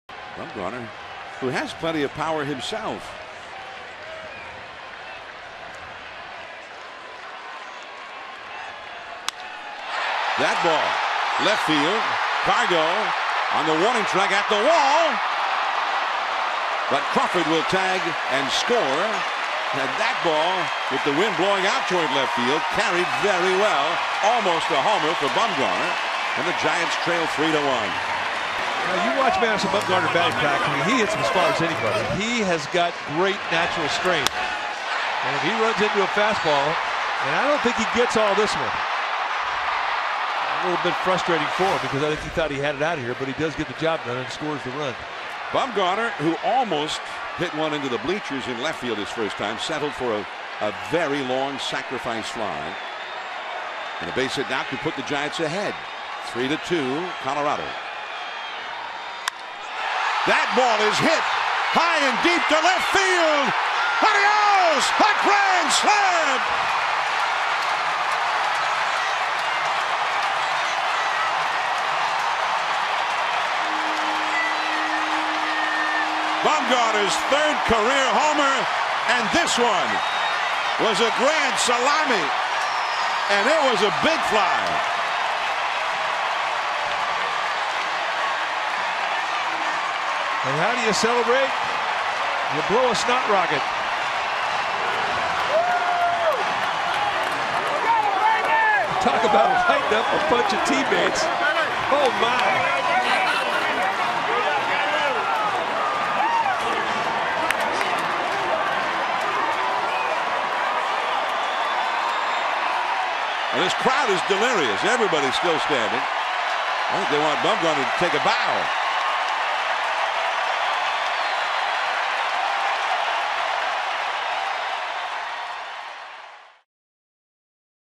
COL@SF: Bumgarner helps own cause with five RBIs
4/11/14: Pitcher Madison Bumgarner helps his own cause with 5 RBIs, including a grand slam, in the Giants 6-5 win over the Rockies